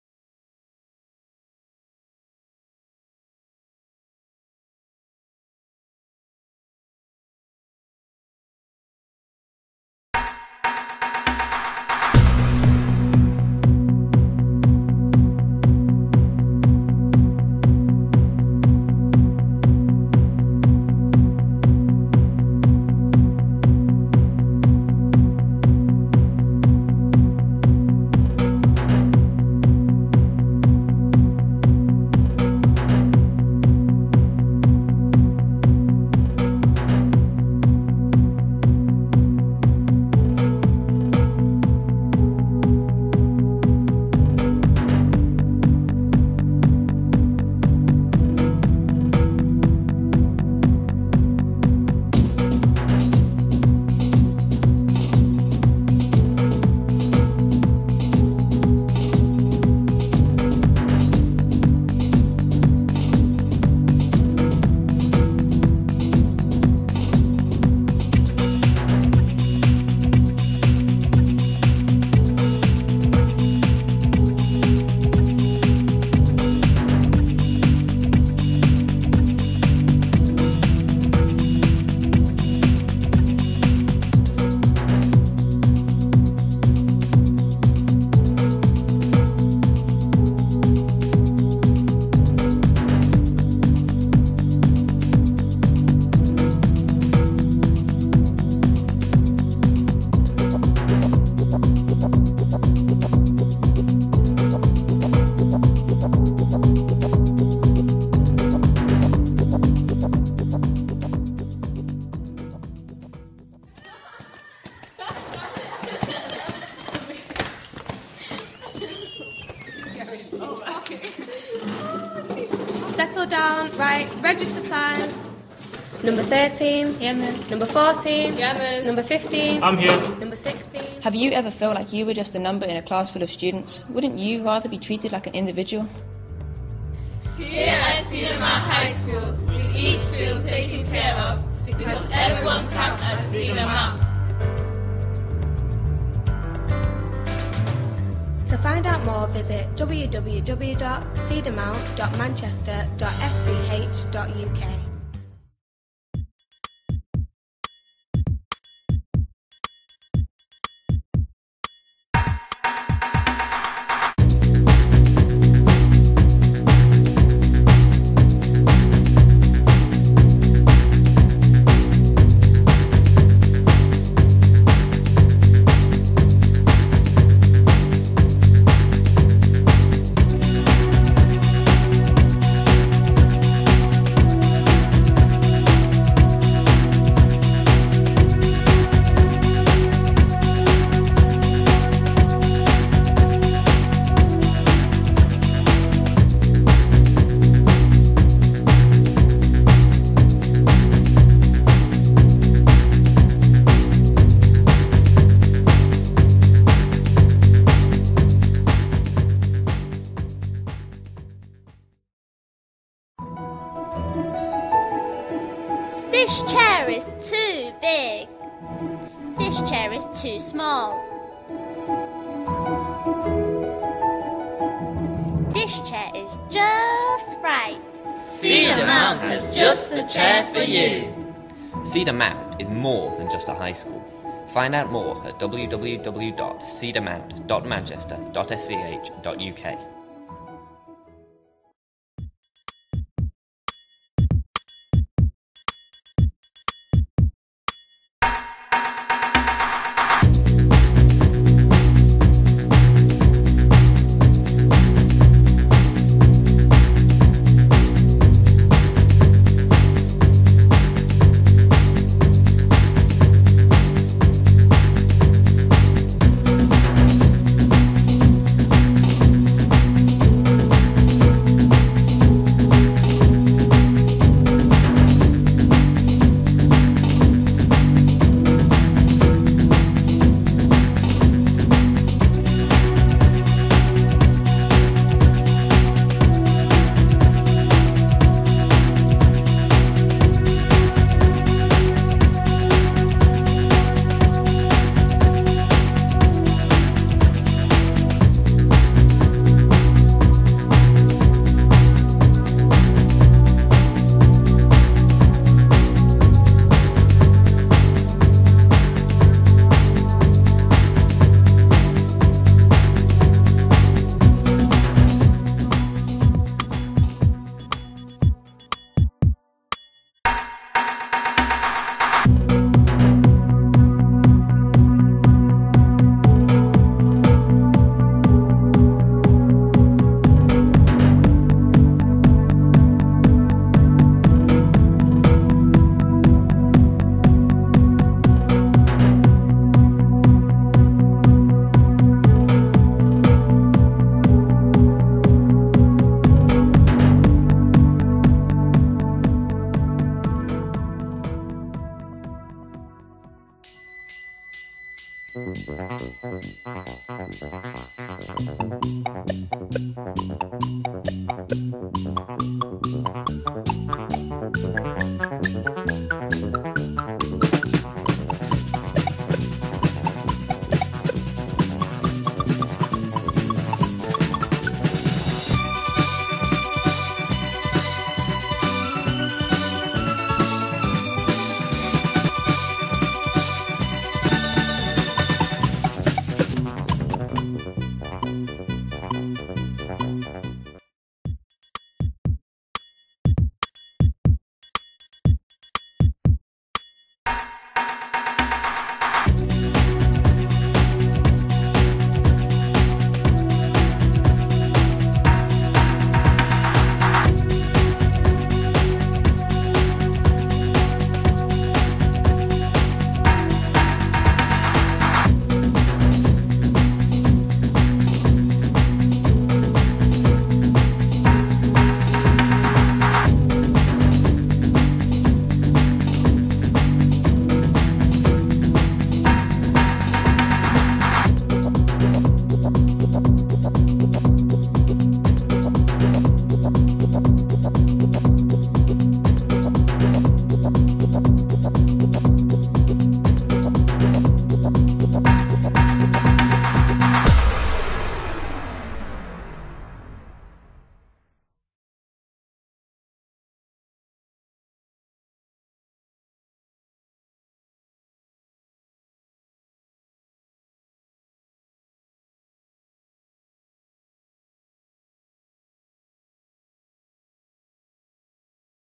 Cedar Mount
Four teams of year ten students compete to come up with the best campaign.
"We would be happy to own these campaigns"
McCann Erickson's project advisors.